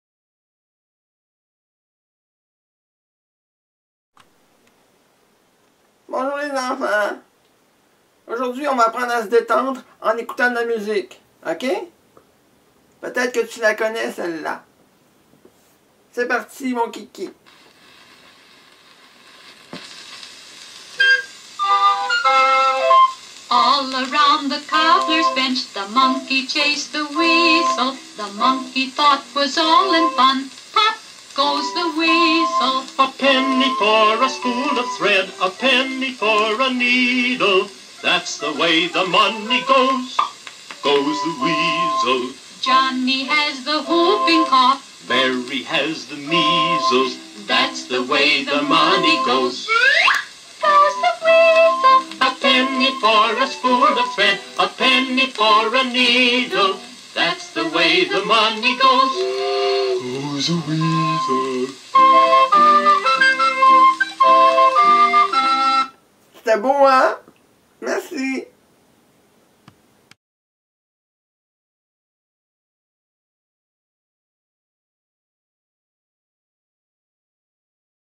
Madame Bobine (belle musique)
Madame Bobine Poirier est un personnage de Mario Bélanger.
Une vieille prof qui enseigne à sa façon.
Merci